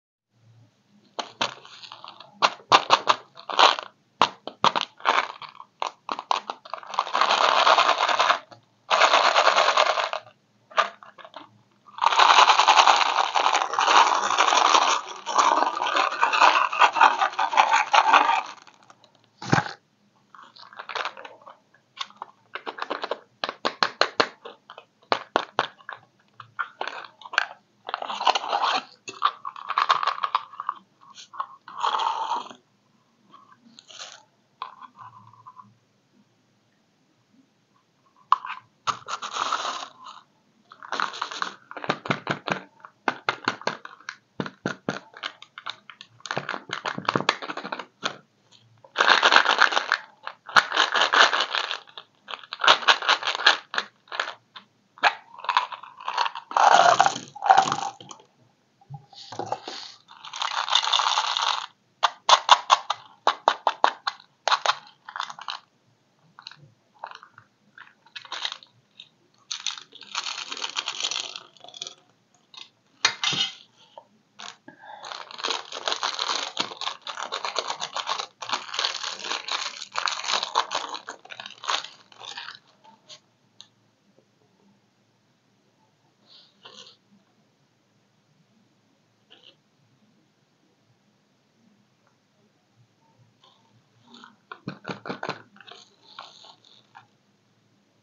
ASMR Unpacking sweets/Scandic, Bubble Gum, Crispy sticks with chocolate cream
Hello, friends!!!

It’s not difficult for you, but it’s nice for me!

sweets machine